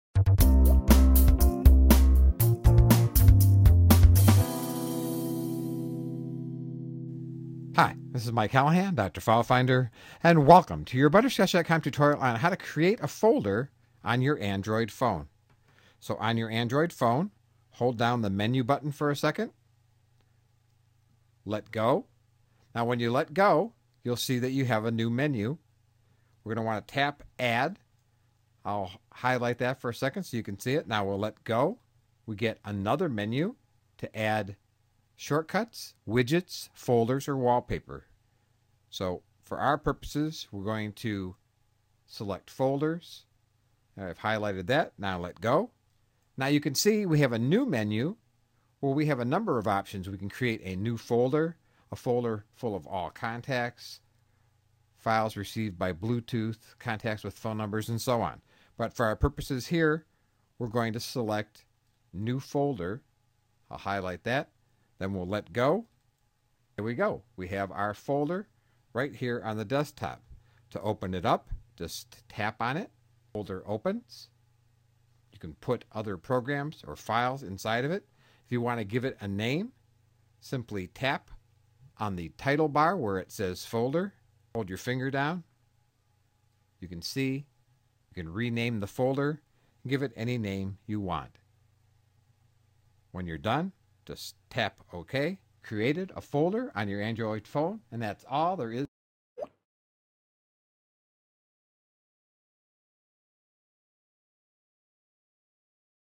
How to create a new folder on your Android phone
If your Android phone's home screen is looking like a bomb hit (perhaps you've been spending too much time downloading new apps from the Android Market) then this is the video tutorial for you. A Internet tutorial by butterscotch.com.